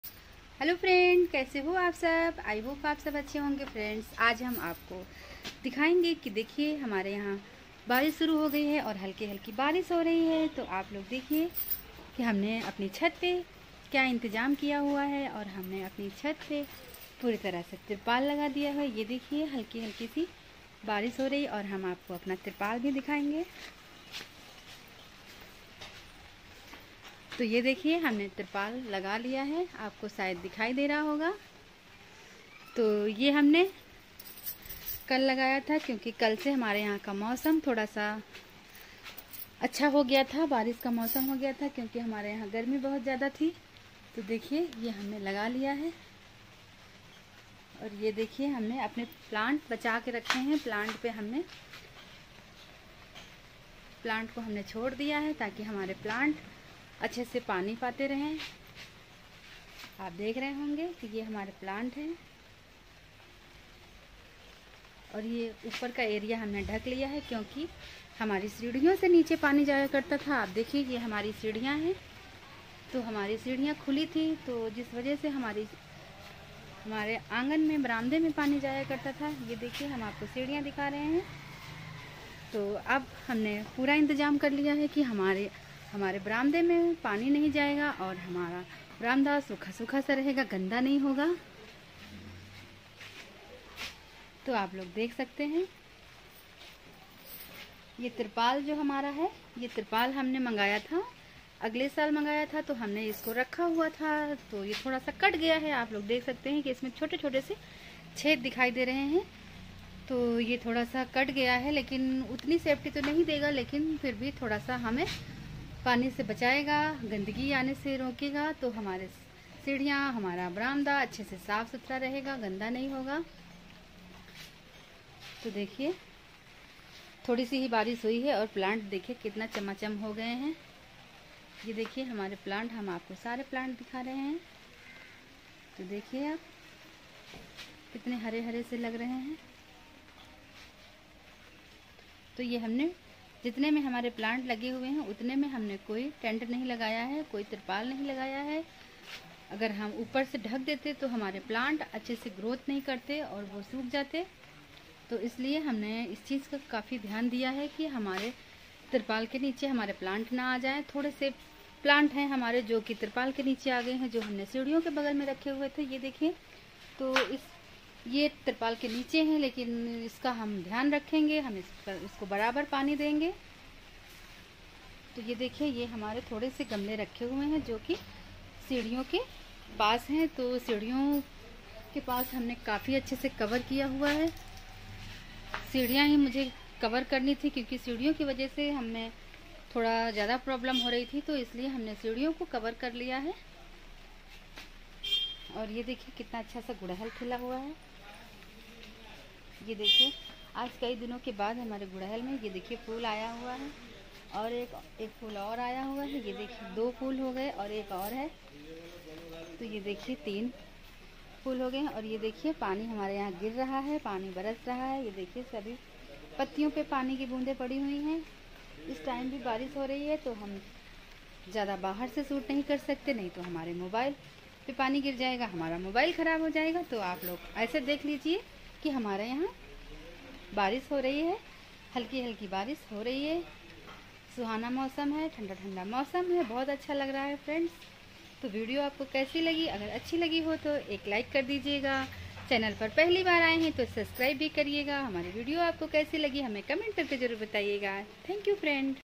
कल से हो रही है लगातार हल्की बारिश हमने पूरी तरह से अपनी छत पर लगाया तिरपाल ll गुड़हल में आए फूल
🙏🙏धन्यवाद् 🙏🙏